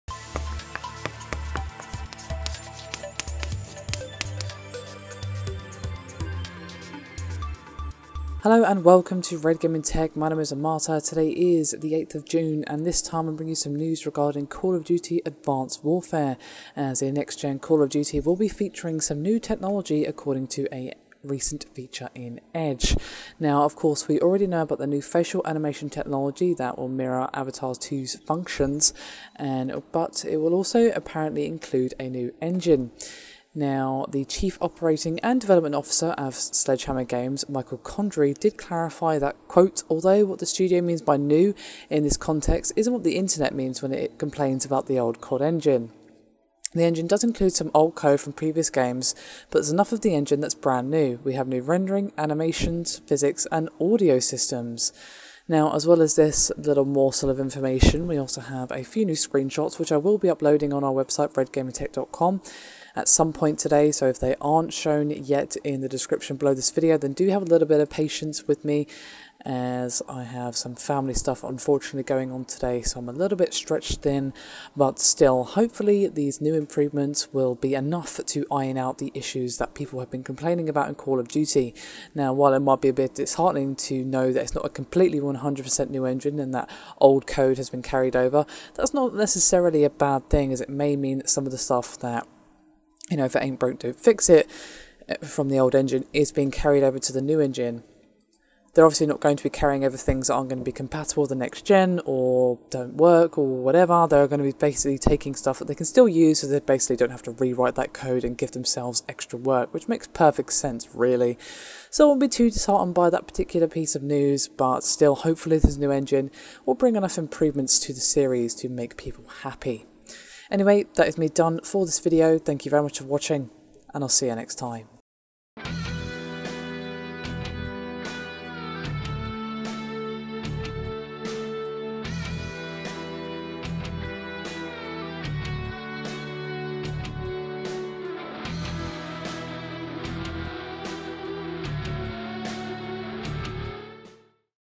Call of Duty Advanced Warfare News - Sledgehammer Talk Improvements & Changes To New CoD Engine
Sledgehammer Games have spoken out on the topic of the Call of Duty Advanced Warfare, and all the changes made to the engine.